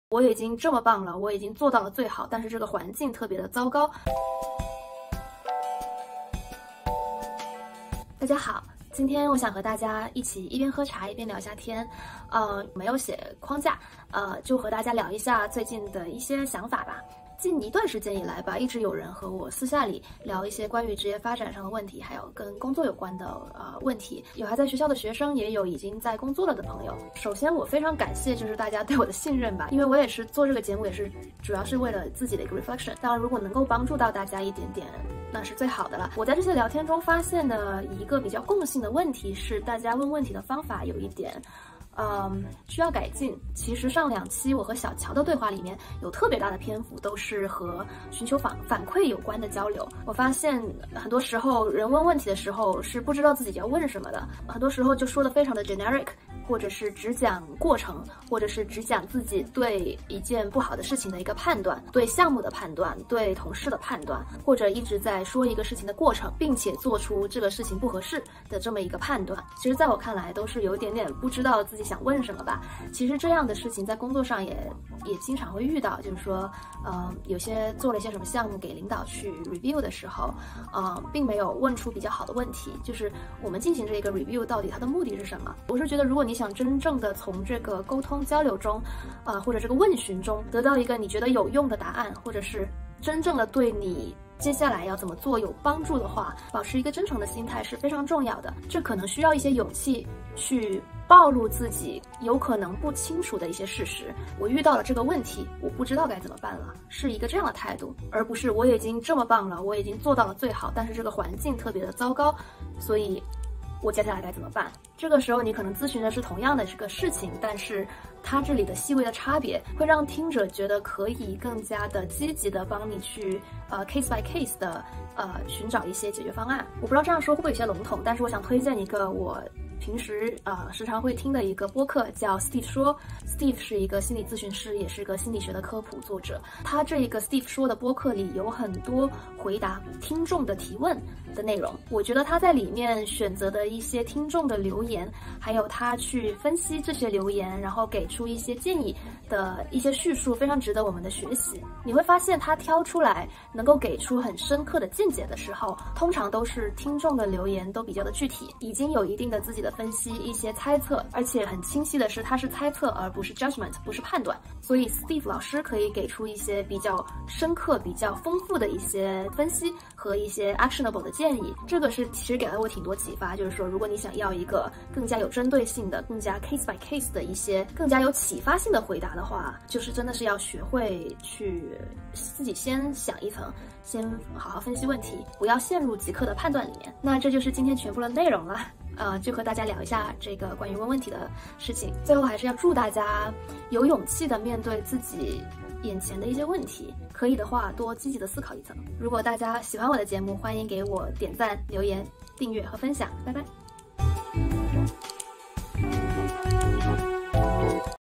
职场#15 好好提问才有可能得到有启发性的回答 | 聊聊提问那些事 Ask the Right Questions In a Right Way
职场#15 好好提问才有可能得到有启发性的回答 | 聊聊提问那些事
Ep. 15 Ask the Right Questions In a Right Way